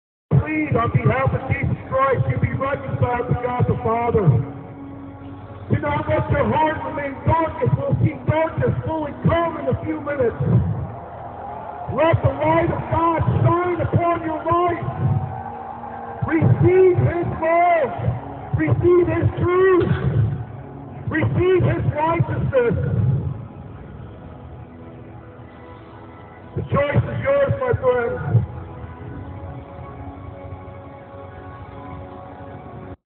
Eclipse outreach at Indianapolis Motor Speedway
Steven preaches at the Indianapolis Motor Speedway